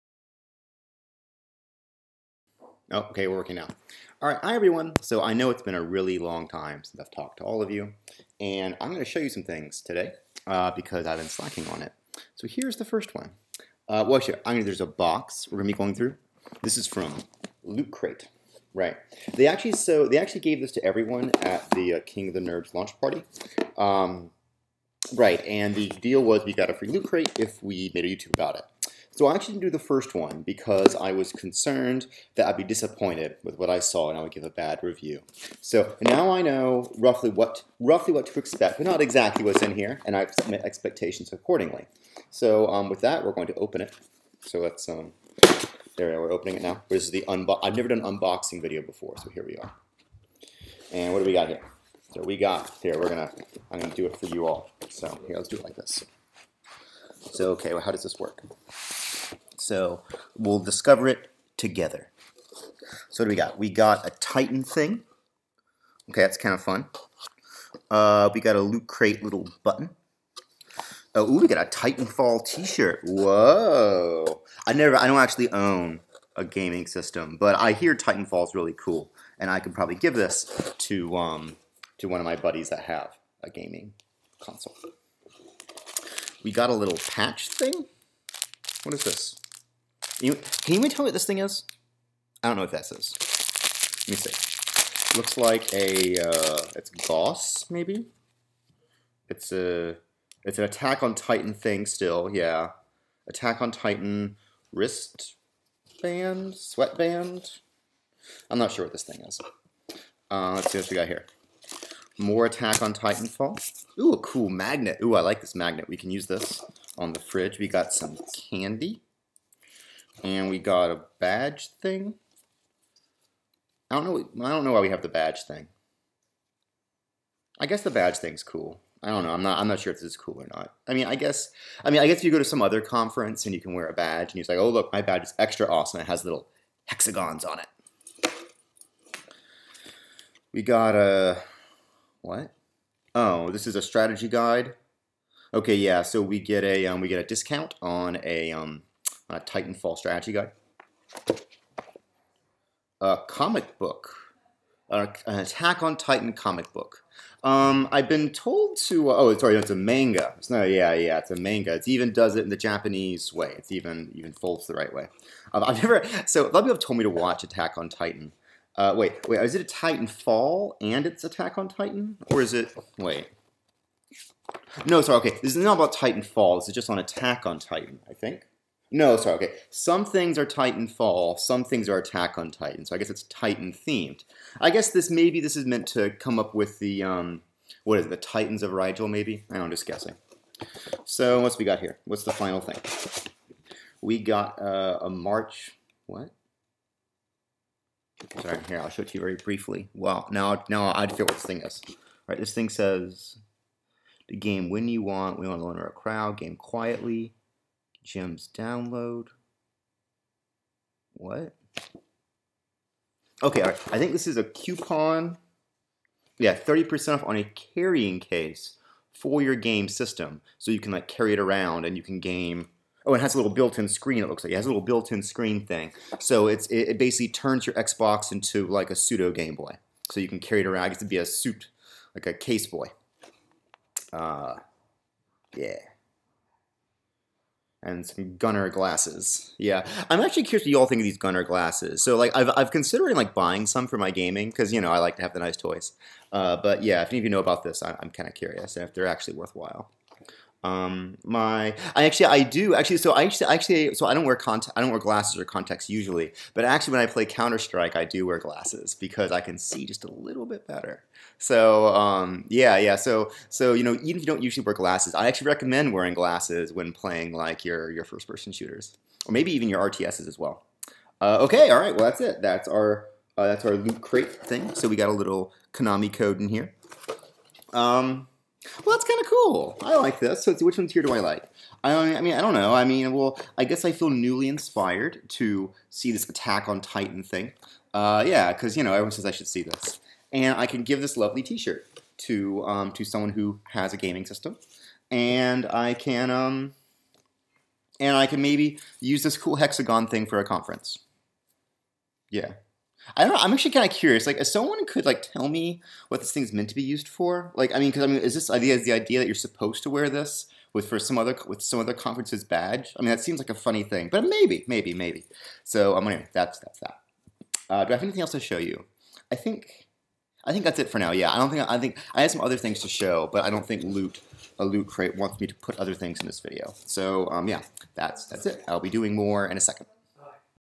LootCrate opening!
I open my second crate from LootCrate!  It's TITAN THEMED!